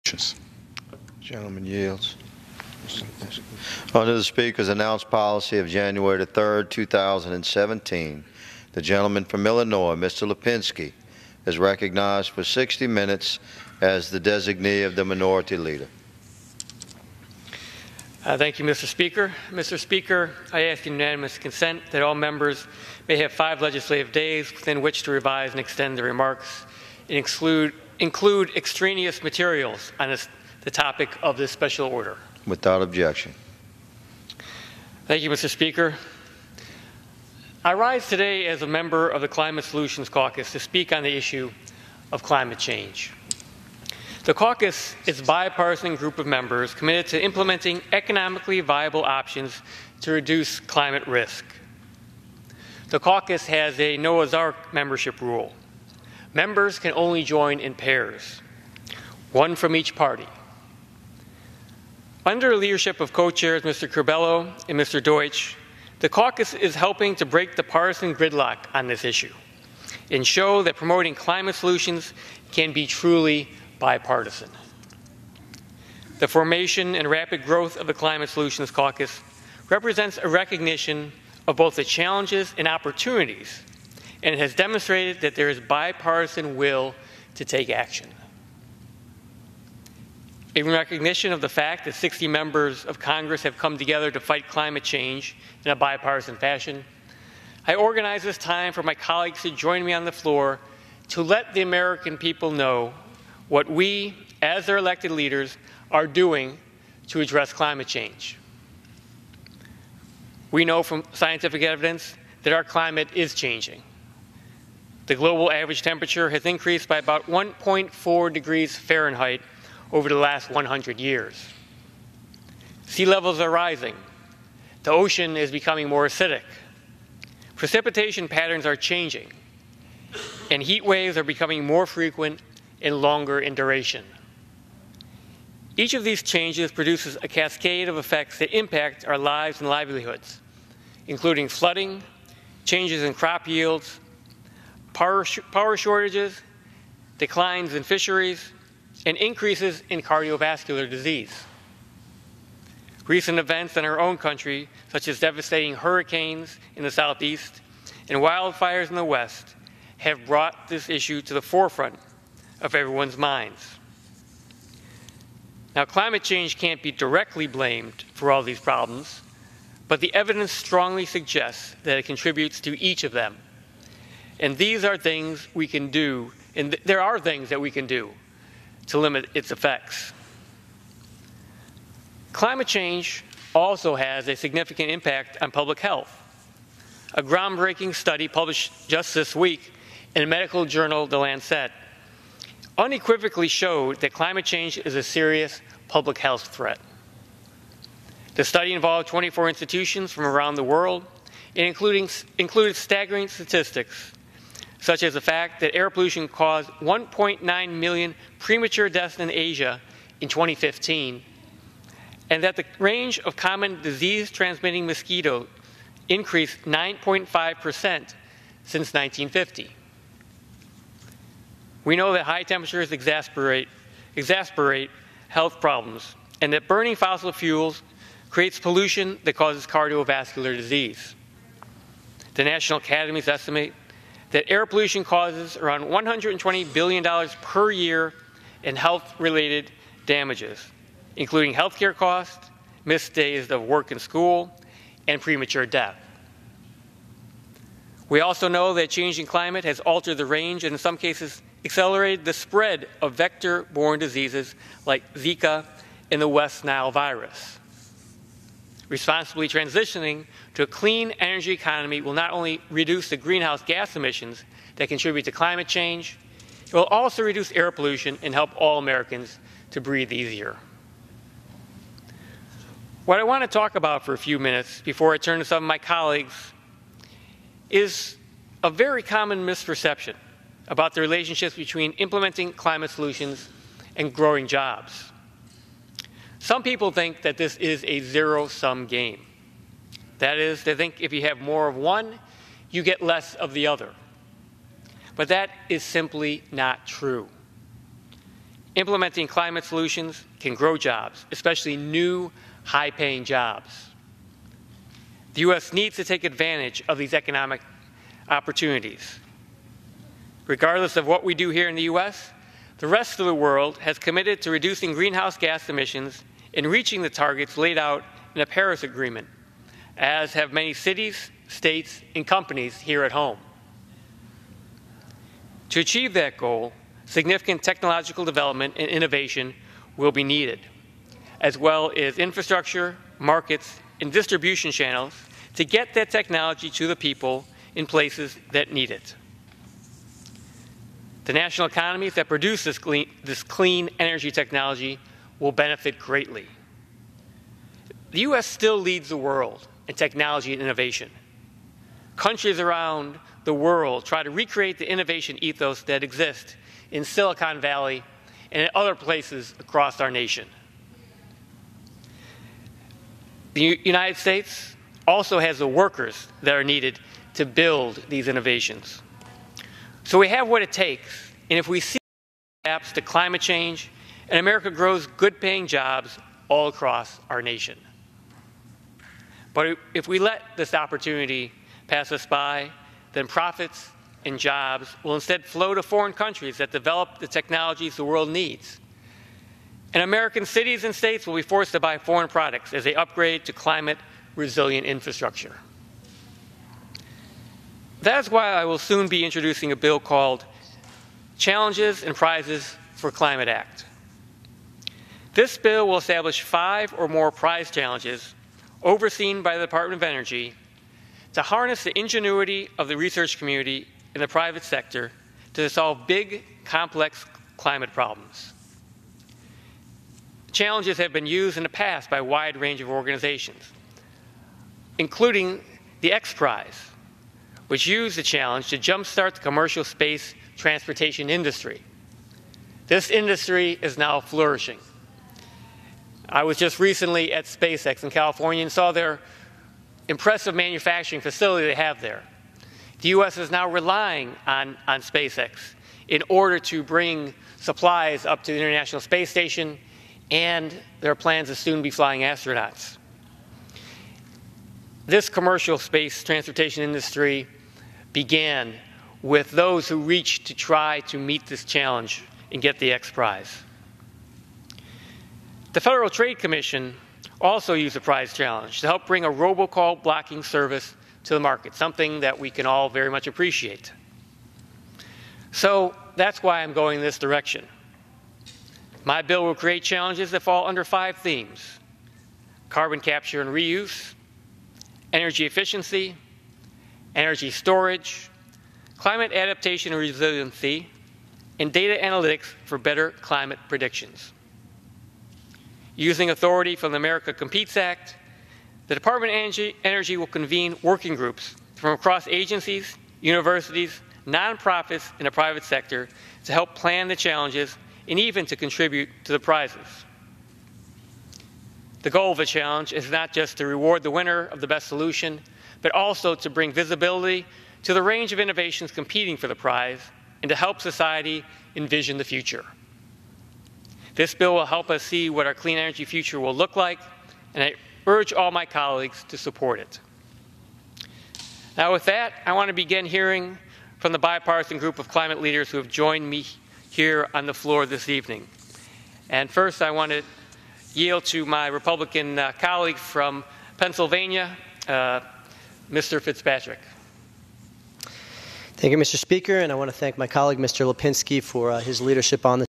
Rep. Dan Lipinski opens floor speeches on climate change
On Nov. 1, 2017, 10 members of the Climate Solutions Caucus gave speeches on the House floor, highlighting the issue of climate change and the need for bipartisan action in Congress. Rep. Dan Lipinski (D-IL), who organized the speeches, was the first to speak.